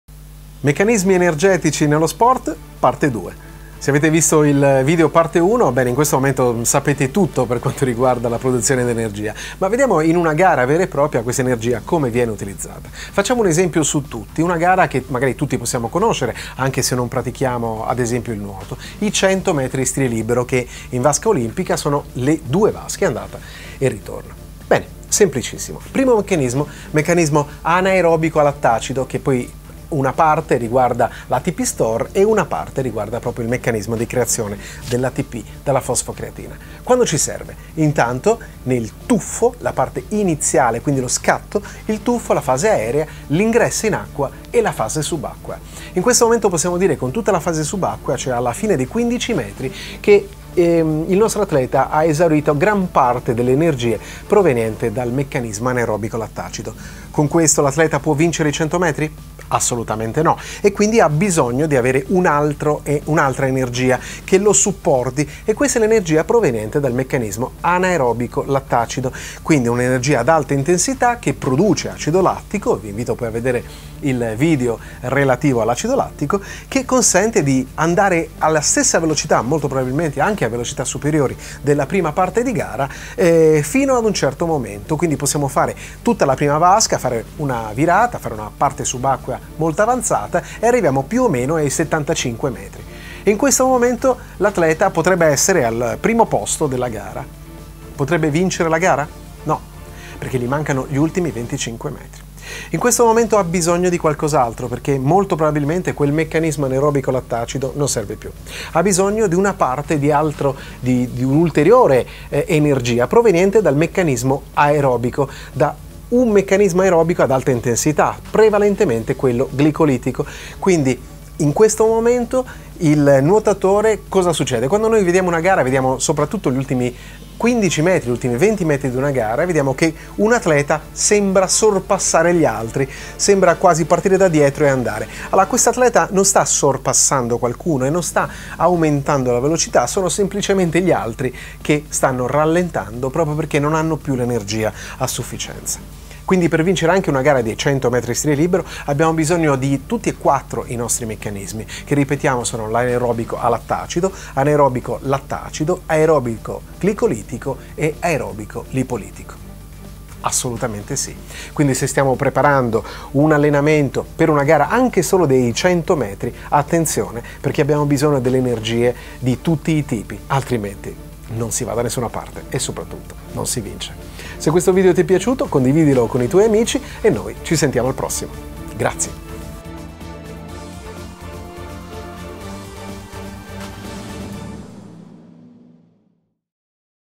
Come avere più energia per la gara nello Sport?
Come creiamo energia durante la gara? Cosa è l'ATP, l'aerobico, l'anaerobico, il glicogeno?

Abbiamo visto nel video 1 cosa è l'energia e come si produce, ora vediamo cosa succede in una gara di velocità o in palestra o in una maratona quando dobbiamo usare tutte le nostre energie per la prestazione.
Come resistere alla fatica? Come ridurre la fatica nella gara? Come andare più veloce?